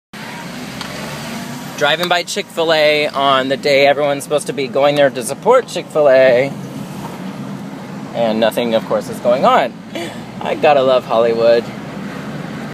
Chic-fil-a Protest Hollywood
The conservatives are out in full support for hate!

;)  Just kidding! Fuck Chic-fil-a!